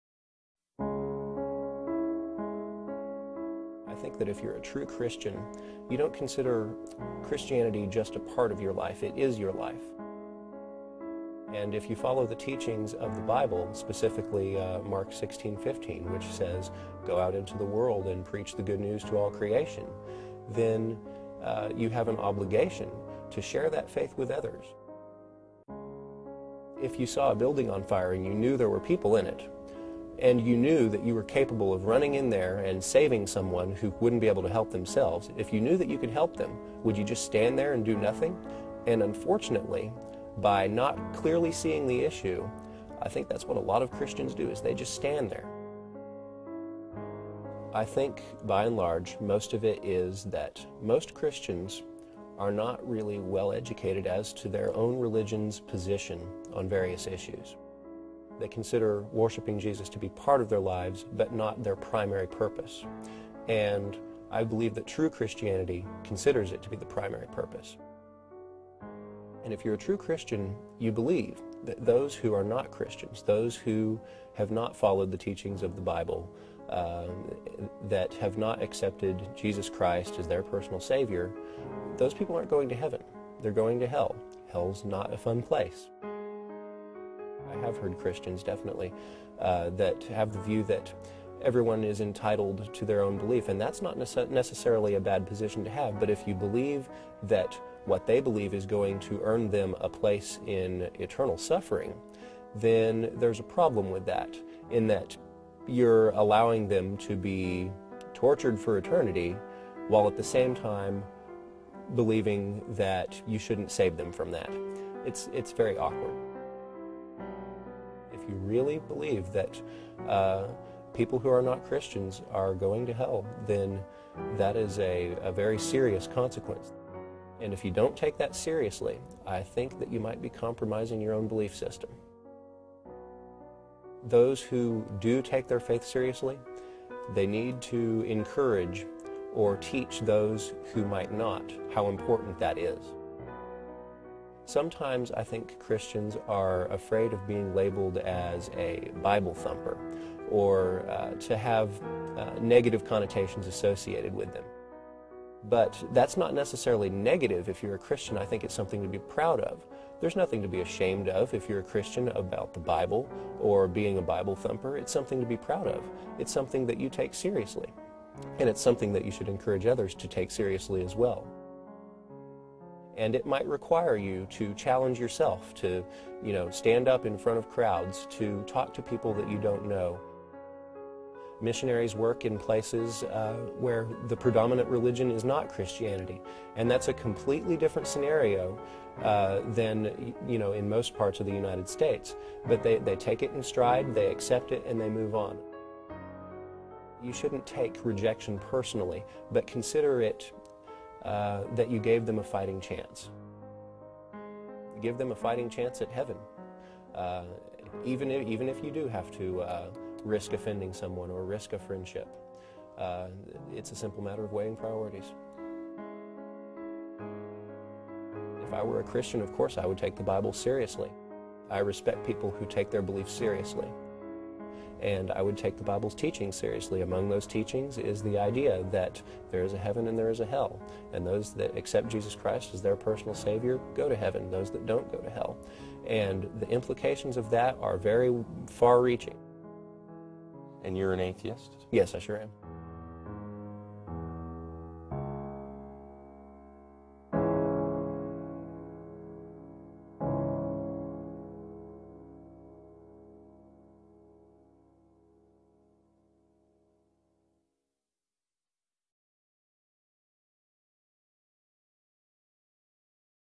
Interview With An Atheist
See if you can guess who is speaking: "If you're a true Christian, you believe that those who are not Christians...that have not accepted Jesus Christ as their personal Savior, those people aren't going to heaven; they're going to hell. And if you follow the teachings of the Bible, specifically Mark 16:15, which says, 'Go out into the world and preach the good news to all creation,' then you have an obligation to share that faith with others." What would you guess this speaker's title to be? Christian Teacher? Minister of the Gospel? Believe it or not, these are the words of a self-professing atheist.